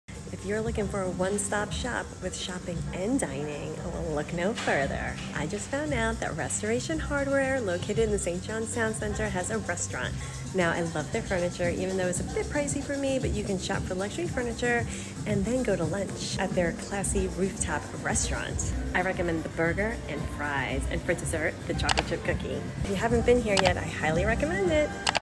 Can a furniture store have the best burgers?🍔
This may surprise you but a well know furniture store (Restoration Hardware) in St Johns Town Center may very well have the best burger. WHAT? Yes i know surprised me too.